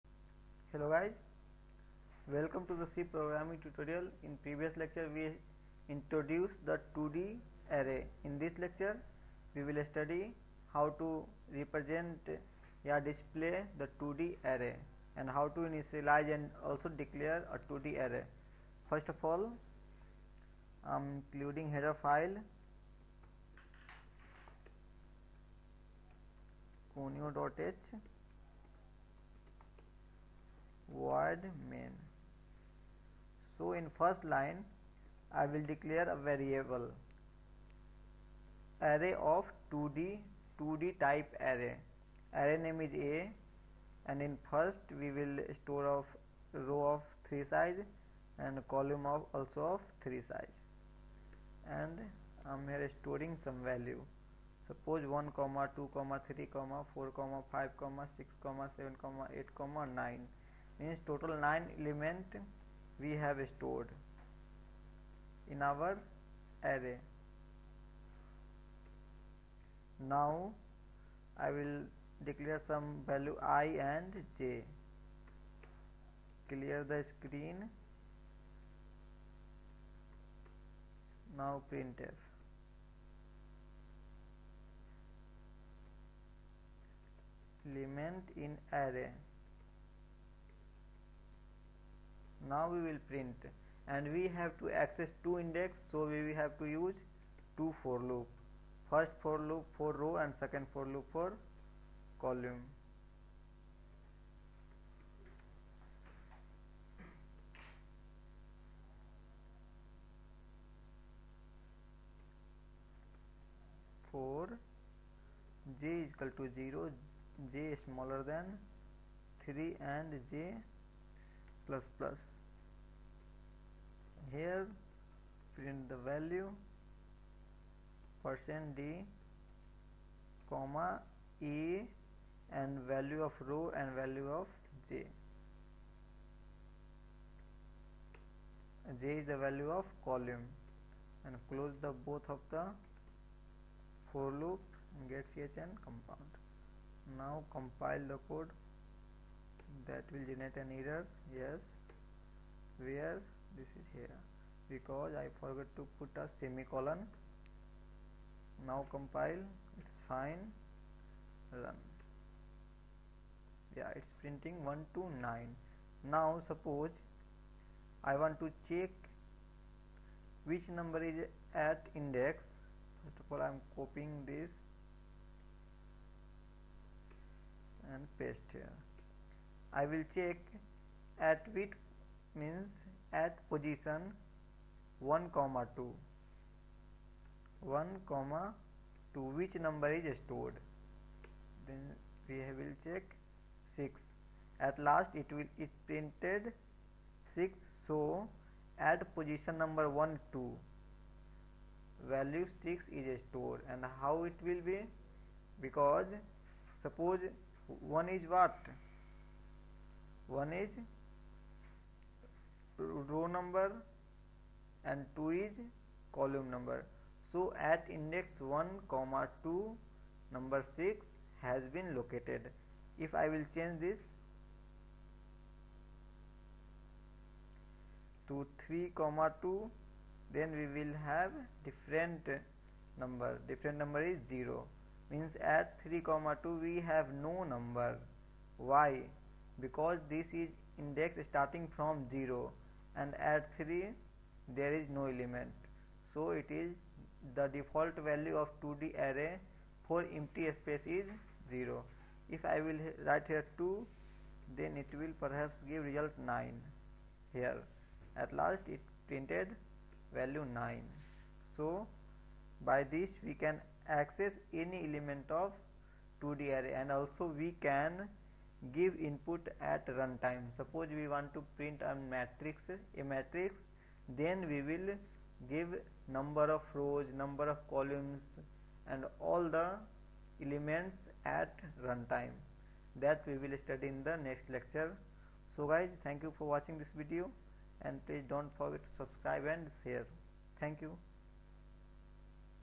c program to declare, initialize and access two dimensional array-C programming tutorial-32
In this lecture we will study how to initialize ,declare and access element of array in c
Programming in c
learn python
python for beginners
python for beginners in 2019
Basic programming
programming for interview